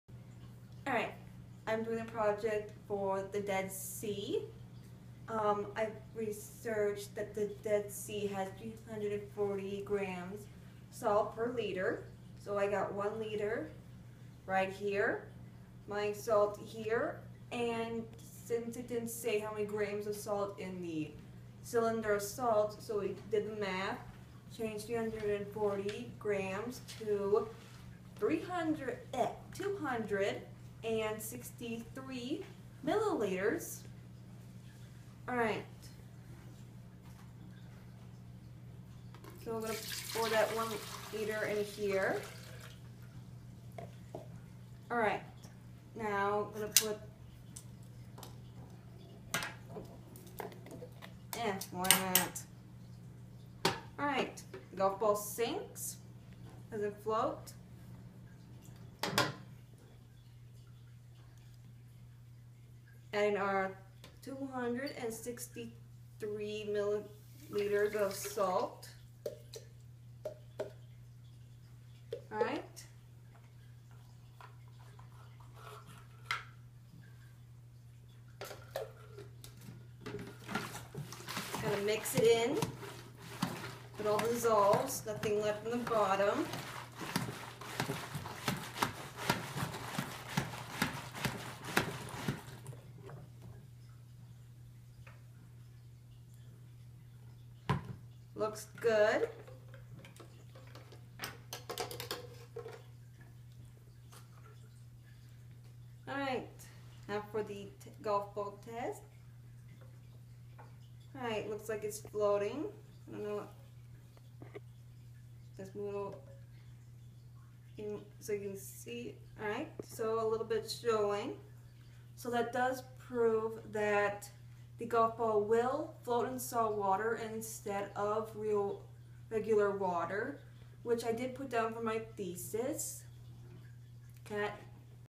Chemistry Project of the Dead Sea
How will a human float in the dead sea?